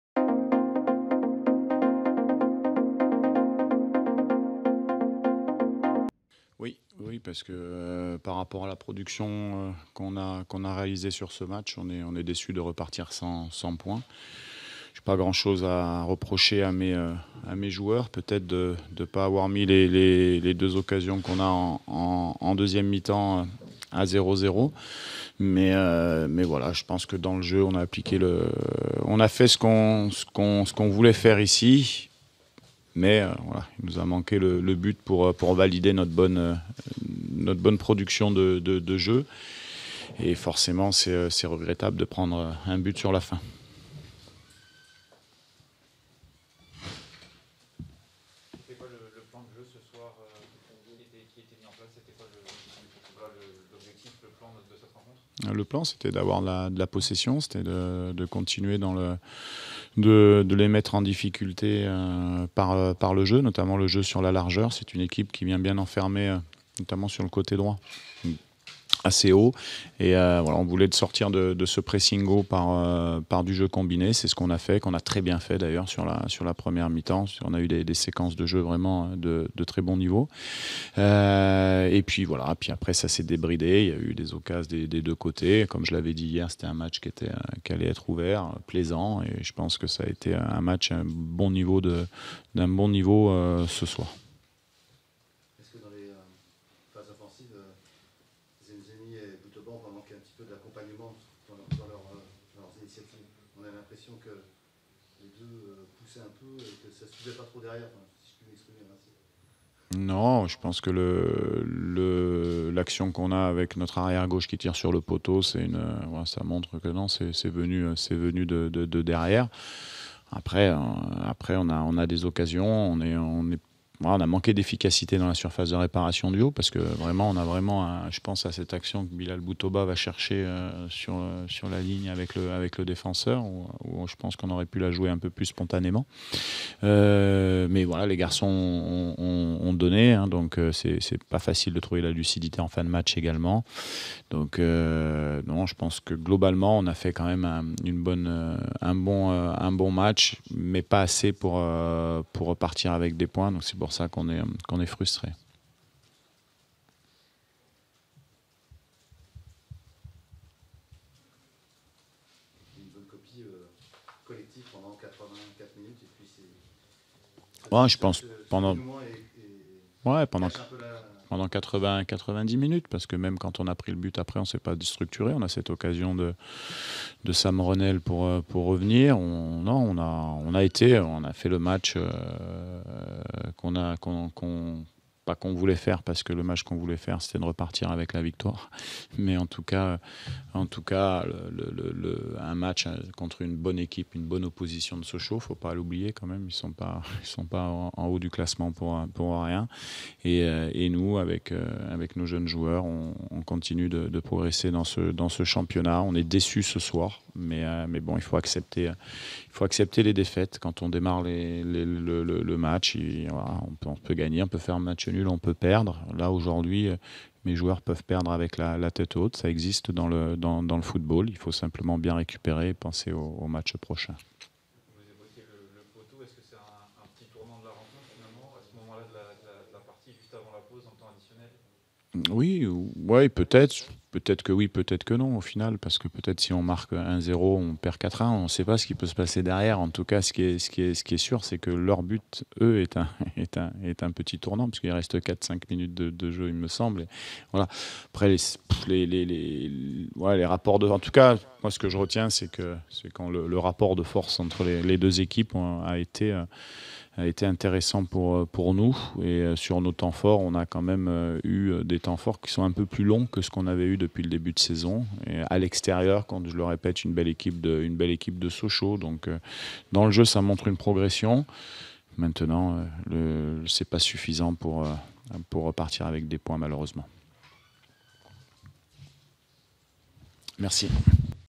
Conférence de presse de Sébastien Desabre après Sochaux/Chamois
Découvrez les mots de coach Desabre après le match FC Sochaux Montbéliard/Chamois Niortais.